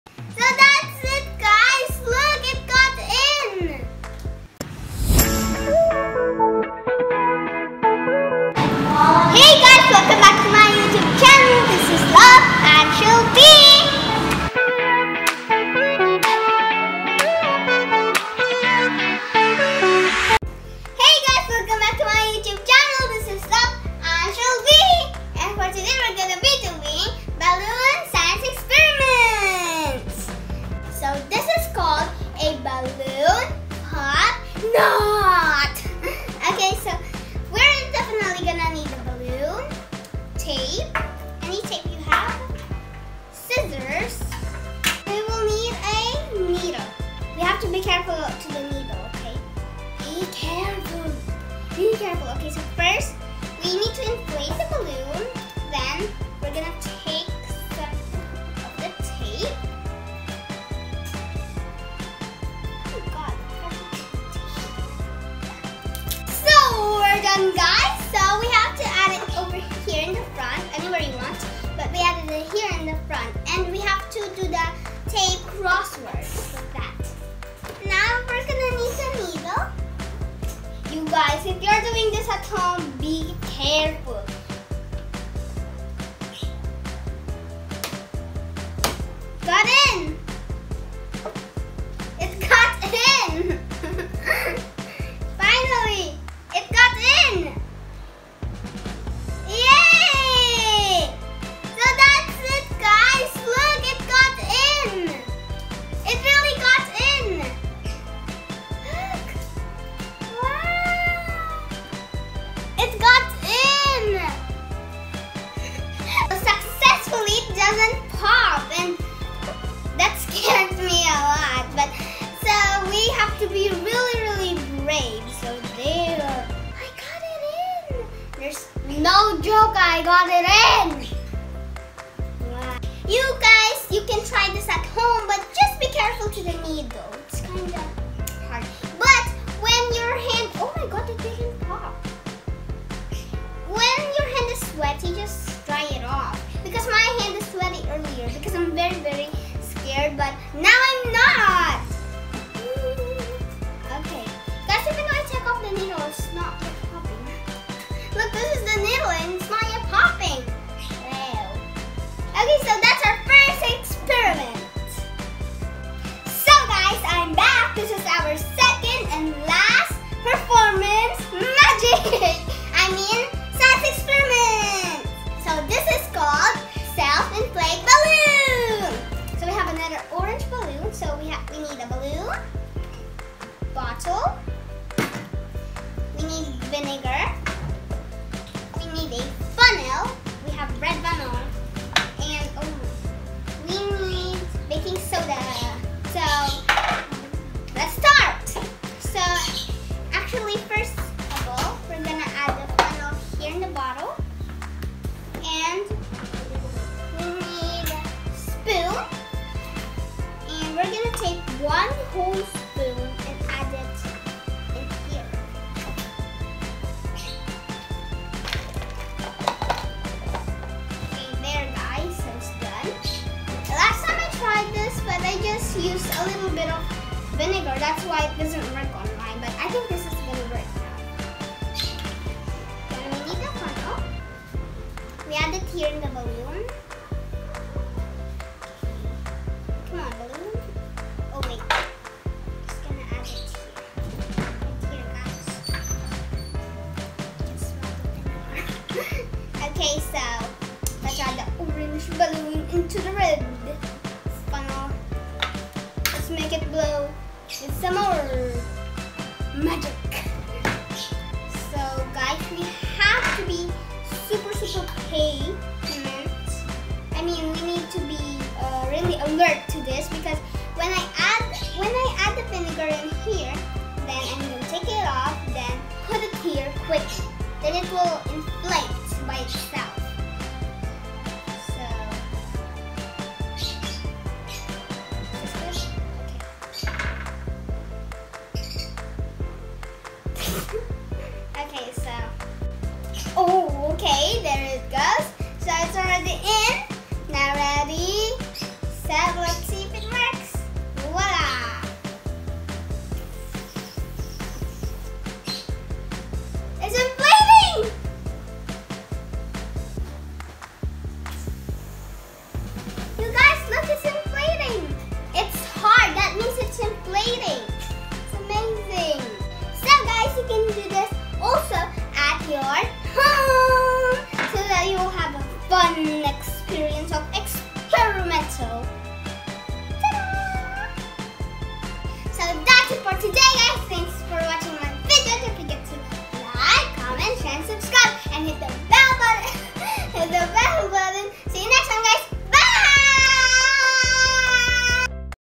Balloon Science Experiment | Anchels Vlogs
This science experiment is so easy to make!
What causes a balloon to pop is called catastrophic crack propagation. This really complicated sounded phrase means that the hole in the balloon widening is what makes it pop, not the fact that air is slowly being let out. When the balloon’s hole gets bigger, it rips, and eventually the balloon pops. In this experiment, the tape slows down this process.

How did this happen? When the vinegar is mixed with baking soda, carbon dioxide gas is produced and this gas causes the balloon to inflate.